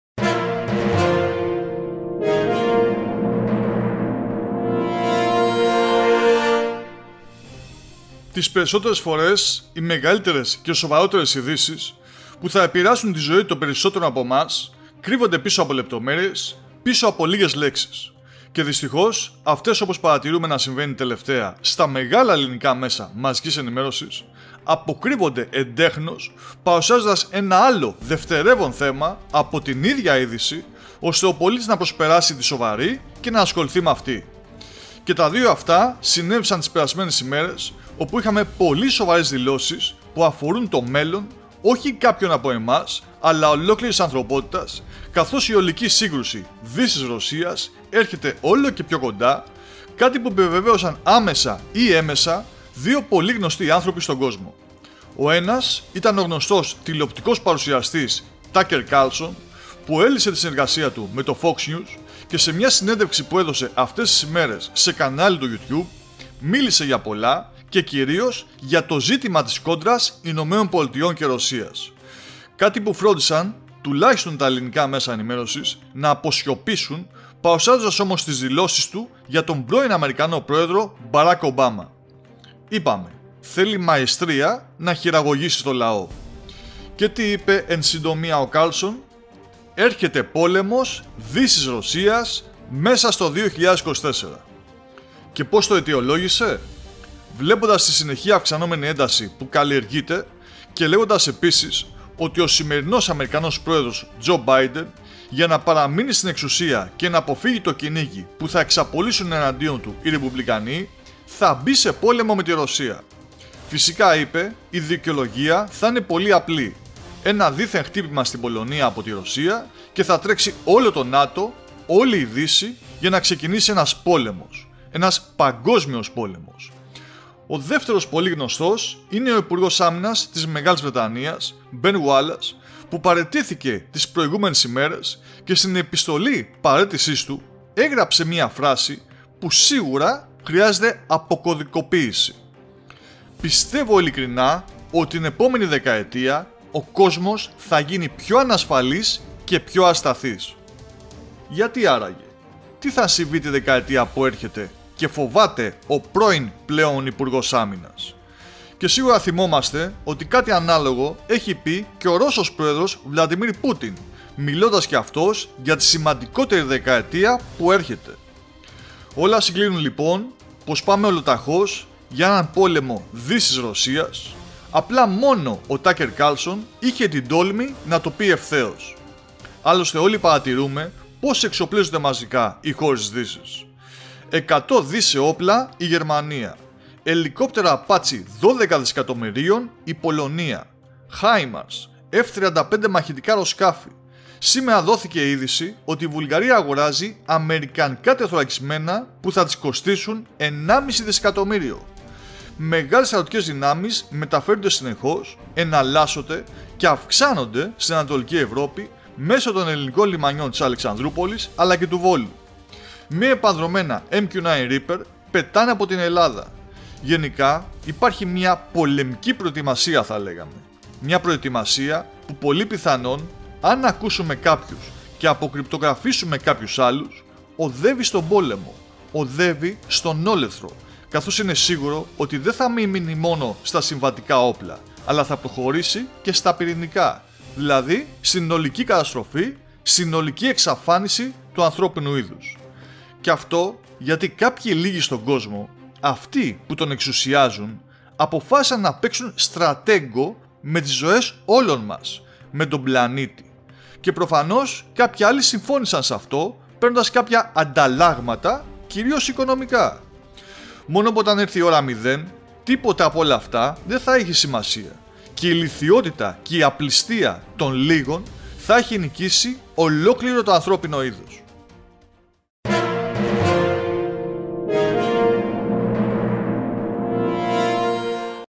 ΗΠΑ - ΡΩΣΙΑ | ΣΟΚ: Έρχεται πόλεμος... - (3.9.2023)[Eng subs]
ΕΛΛΑΔΑ. ΤΟΥΡΚΙΑ. ΚΥΠΡΟΣ. ΡΩΣΙΑ. ΗΠΑ. ΟΥΚΡΑΝΙΑ. ΓΕΡΜΑΝΙΑ. ΓΑΛΛΙΑ. ΒΡΕΤΑΝΙΑ. ΚΙΝΑ. ΙΝΔΙΑ.
Τις περισσότερες φορές οι μεγαλύτερες και σοβαρότερες ειδήσεις, που θα επηρεάσουν την ζωή των περισσότερων από εμάς, κρύβονται πίσω από λεπτομέρειες, πίσω από λίγες λέξεις.
Και δυστυχώς αυτές όπως παρατηρούμε να συμβαίνει τελευταία στα μεγάλα Ελληνικά μέσα μαζικής ενημέρωσης, αποκρύβονται εντέχνως, παρουσιάζοντας ένα άλλο δευτερεύον θέμα, από την ίδια είδηση, ώστε ο πολίτης να προσπεράσει την σοβαρή και να ασχοληθεί με αυτή.
Και τα δυο αυτά, συνέβησαν τις περασμένες ημέρες, όπου είχαμε πολύ σοβαρές δηλώσεις που αφορούν το μέλλον, όχι κάποιων από εμάς, αλλά ολόκληρης της ανθρωπότητας, καθώς η ολική σύγκρουση Δύσης - Ρωσίας, έρχεται όλο και πιο κοντά, κάτι που επιβεβαίωσαν άμεσα ή έμμεσα, δυο πολύ γνωστοί άνθρωποι στον κόσμο.
Ο ένας, ήταν ο γνωστός τηλεοπτικός παρουσιαστής Tάκερ Κάρλσον, που έλυσε τη συνεργασία του με το FOX News και σε μια συνέντευξη που έδωσε αυτές τις ημέρες, σε κανάλι του Youtube, μίλησε για πολλά και κυρίως για το ζήτημα της κόντρας ΗΠΑ και Ρωσίας.
Κάτι που φρόντισαν, τουλάχιστον τα Ελληνικά μέσα ενημέρωσης, να αποσιωπήσουν, παρουσιάζοντας όμως τις δηλώσεις του για τον πρώην Αμερικανό πρόεδρο, Μπαράκ Ομπάμα.
Είπαμε θέλει μαεστρία να χειραγωγήσεις τον λαό.
Και τι είπε εν συντομία, ο Κάρλσον;
Έρχεται πόλεμος Δύσης - Ρωσίας, μέσα στο 2024.
ΠΗΓΕΣ: Adam Carolla (ΥouTube Channel), capital.gr, news.sky.com

GREECE. TURKEY. CYPRUS. RUSSIA. USA. UKRAINE. GERMANY. FRANCE. BRITAIN. CHINA. INDIA.
Most of the time the biggest and most serious news, which will affect the lives of most of us, are behind details, behind a few words.
And unfortunately these, as we have noticed happening lately in the major Greek mass media, are hidden very clever, presenting another secondary issue, from the same news, so that the citizen bypass the serious one and see only this.
Both of these have happened in the past few days, where we have had very serious statements regarding the future, not of some of us, but of all humanity, as the total confrontation of the West vs Russia, comes closer and closer, which they directly or indirectly confirmed, two very known people in the world.
The first was  the well-known TV presenter Tucker Carlson, who ended his collaboration with FOX News and in an interview he gave these days, on a YouTube channel, he talked about many things and mainly about the issue of the US-Russia rivalry.
Something that, at least the Greek media, took care to keep quiet, presenting his statements about the former US president, Barack Obama.
We have already said it to you. It takes mastery to manipulate the people.
And what did Carlson say in brief?
A West-Russia war is coming in 2024.
Sources: Adam Carolla (ΥouTube Channel), capital.gr, news.sky.com

Except:
*1) (Photos and videos from Pexels, Pixabay and Unsplash)
Β) («Ben Wallace Official Cabinet Portrait, September 2022 (cropped)» Simon Dawson / No 10 Downing Street, OGL 3 via Wikimedia Commons
*3) ( Μultiple Photos and Videos from US DoD, PUBLIC DOMAIN downloaded from dvidshub)